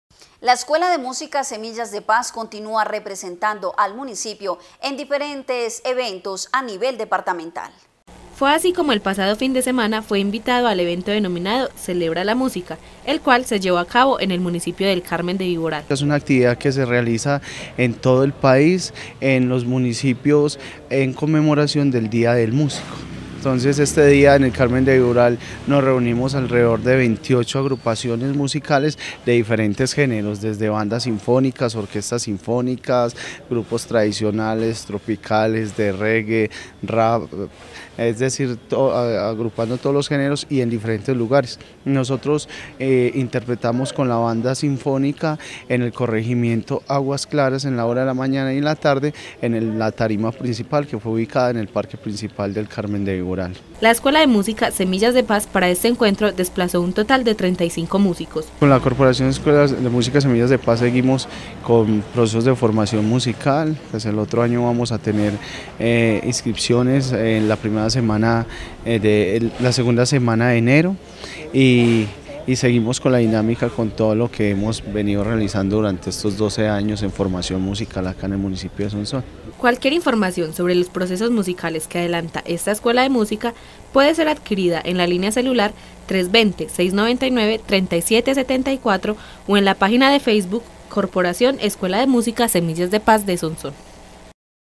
“SEMILLAS DE PAZ” PARTICIPÓ EN ENCUENTRO REGIONAL
La Escuela de Música “Semillas de Paz” continúa representando al municipio en diferentes eventos del ámbito departamental.